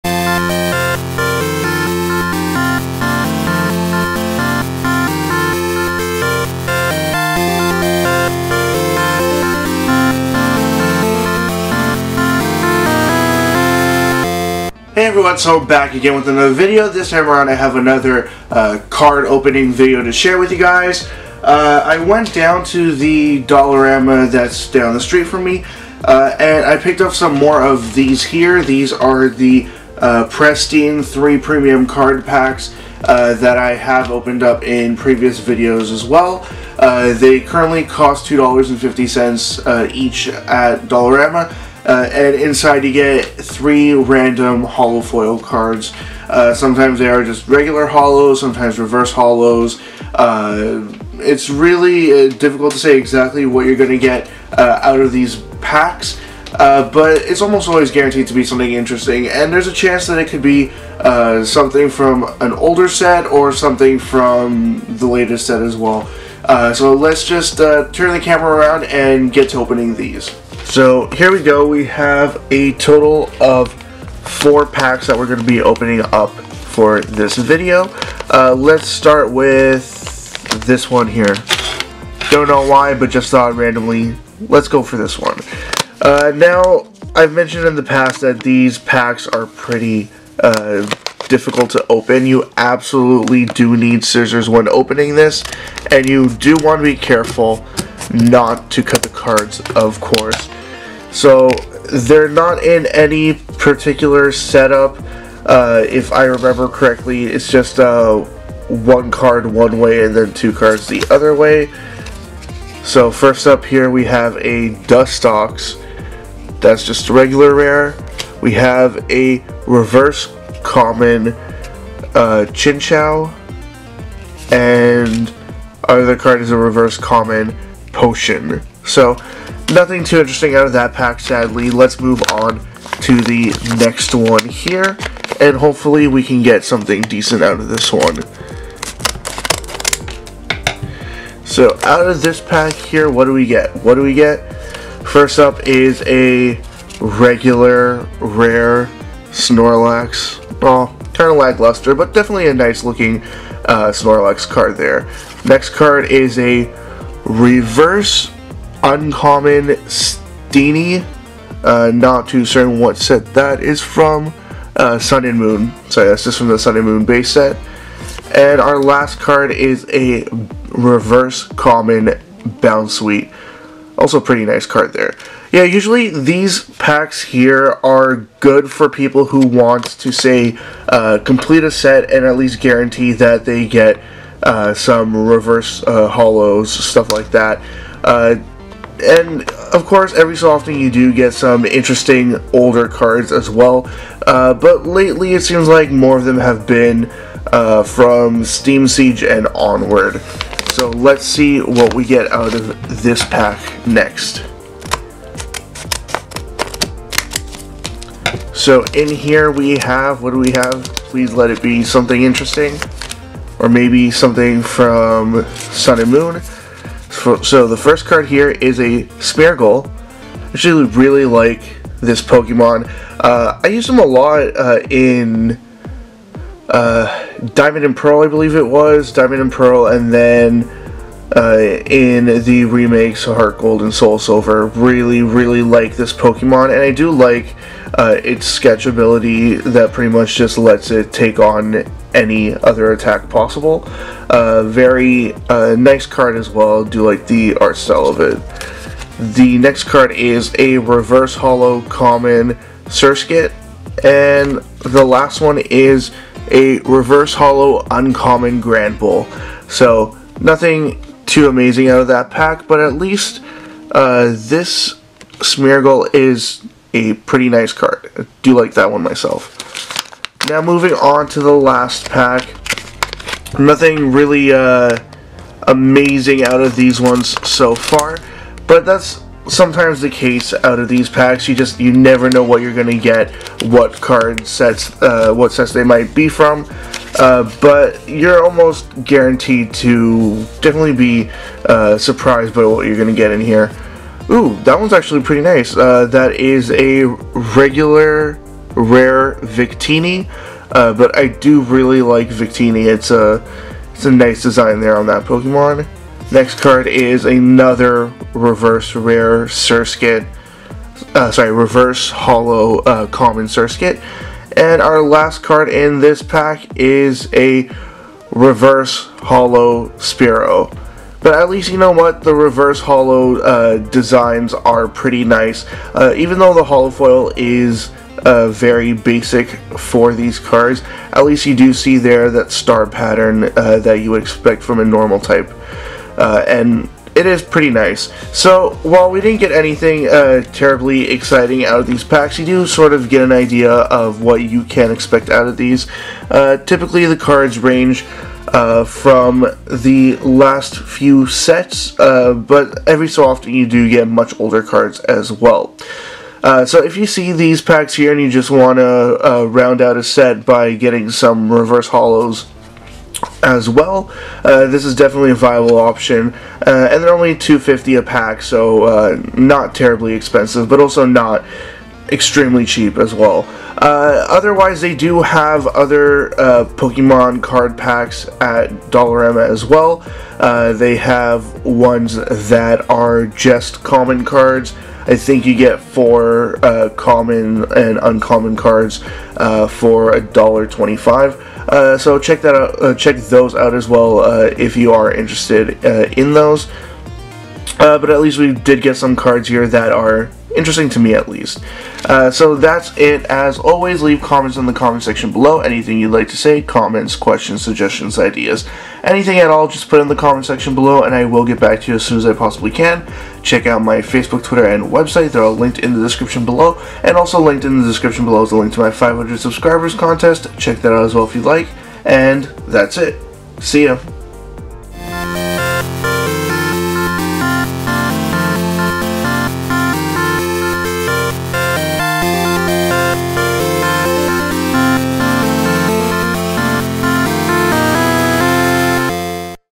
Card Opening Time! with Dragon Dude Collecting: Dollarama Pokemon Cards #4
Trying another Pokemon card pack opening. Let me know what you think.